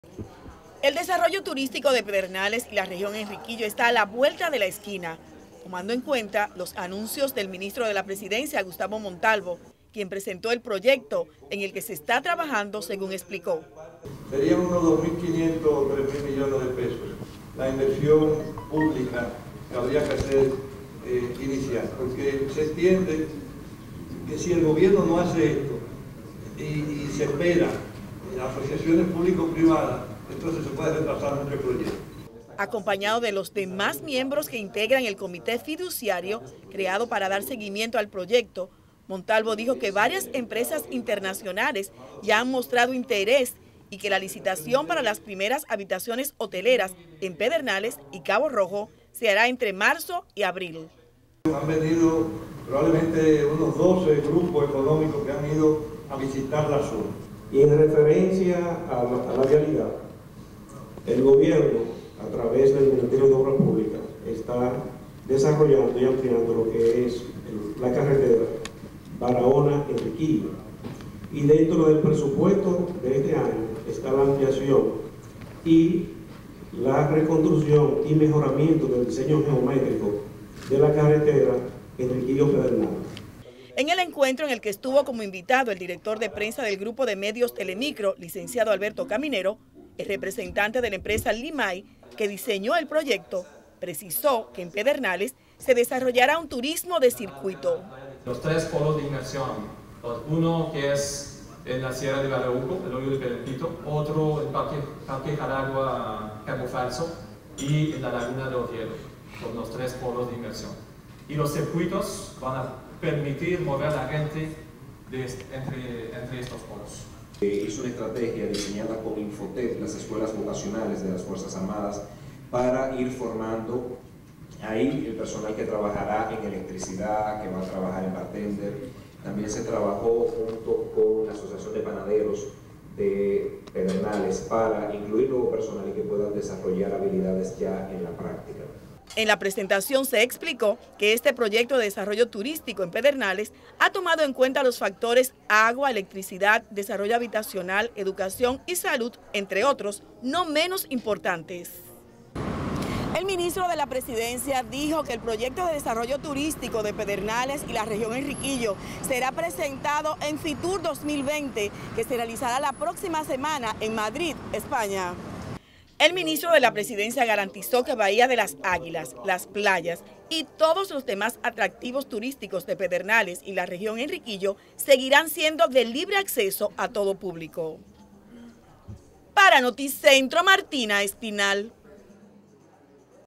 Invertirán 3,000 millones de pesos en primera etapa de desarrollo de Pedernales
Gobierno Dominicano invertirá tres mil 500 millones de pesos, en la primera etapa del proyecto de desarrollo Turístico de Pedernales y toda la Región Enriquillo
Martina Espinal estuvo en la presentación del proyecto y nos amplía